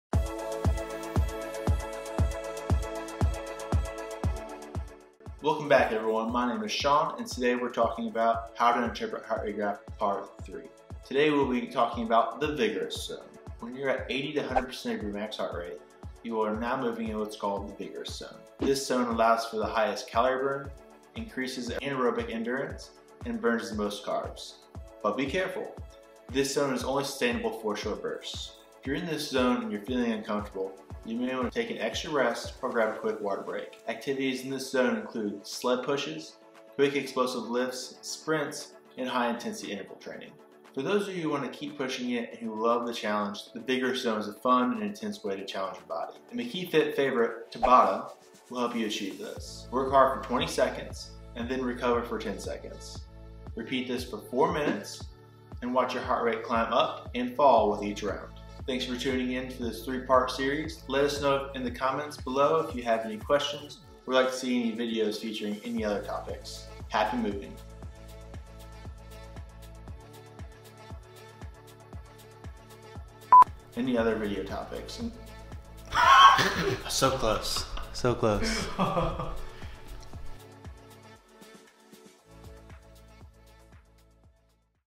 How to interpret a heart rate graph: The Vigorous Zone!
Welcome back everyone. My name is Sean and today we’re talking about how to interpret a heart rate graph, part 3. Today we will be talking about the Vigorous zone.

Last but not least, when you’re at 80%-100% of your heart rate max, you are now moving in what’s called, “the vigorous zone”. This zone allows for the highest calorie burn, increases anaerobic endurance, and burns the most carbs. But be careful, this zone is only sustainable for short bursts.

If you’re in this zone and feeling uncomfortable, you may want to take some extra rest, or grab  a quick water break. Activities in this zone include sled pushes, quick explosive lifts, sprints, and high intensity interval training.

For those who want to keep pushing it and who love the challenge, the vigorous zone is a fun and intense way to challenge your body. A McKeeFIT favorite, “Tabata” will help you achieve this. Work hard for 20 seconds, and then recover for 10 seconds. Repeat this for 4 minutes. Watch your heart rate climb up and fall with each round.

Thanks for tuning in for this 3 part series.

Happy moving!